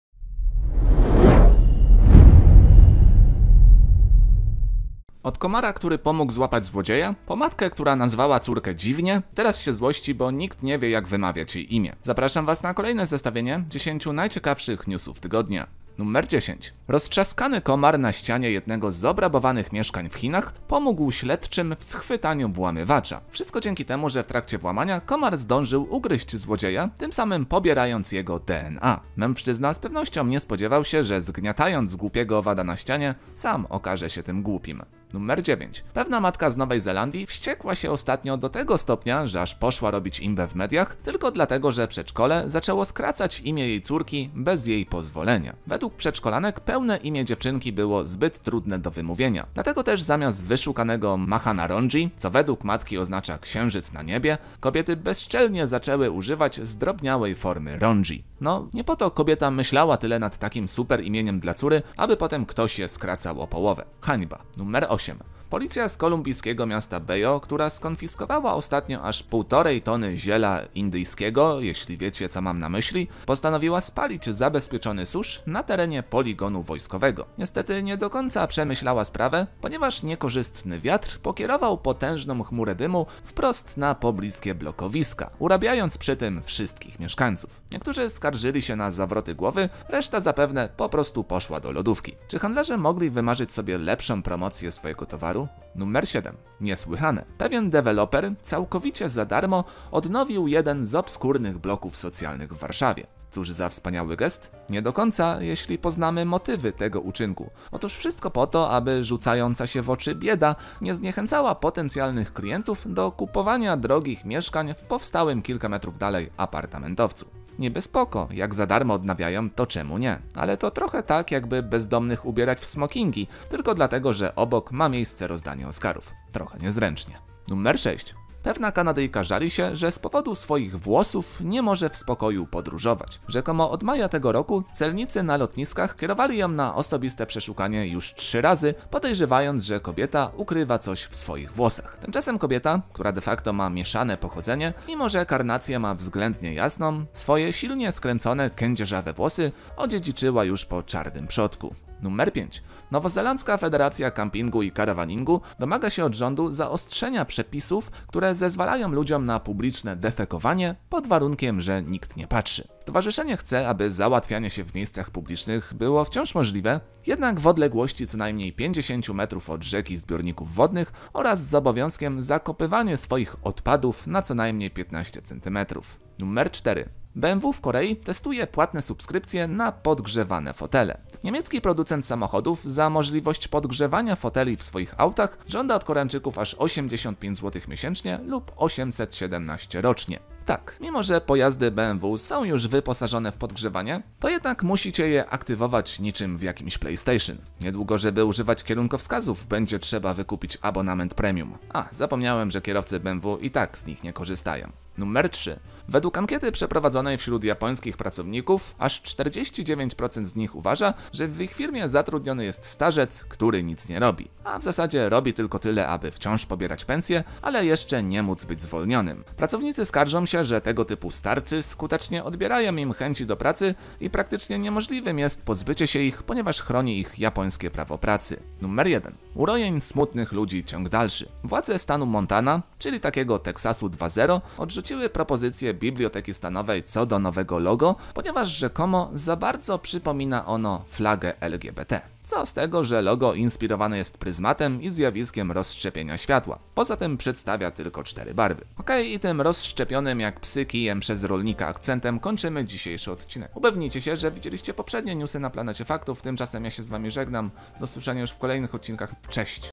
10 najciekawszych newsów tygodnia (19/07/2022)
Od komara, który pomógł złapać złodzieja, po matkę, która nazwała córkę dziwnie i teraz się złości bo nikt nie wie jak wymawiać jej imię, zapraszam was na kolejne zestawienie 10 najciekawszych newsów tygodnia.

Muzyka w tle:
(biblioteka YouTube)

Pozdrawiam,
Jacek Makarewicz